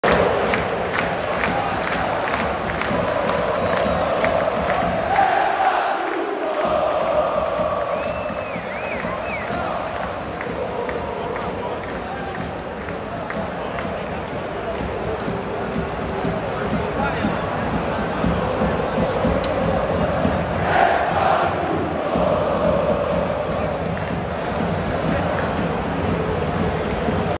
FCSB 1 - 0 CFR Cluj 06.08.2023 Pe Stadionul Ghencea | Ne faci un gol !
FCSB a disputat partida din cadrul etapei a 4-a a Superligii in compania celor de la CFR Cluj, castigand cu scorul de 1 - 0  prin golul marcat de Andrea Compagno din penalty. Astfel, după patru etape, FCSB este lider în clasament, cu 12 puncte, în timp ce CFR Cluj este pe locul patru,, la egalitate cu Sepsi, cu șapte puncte.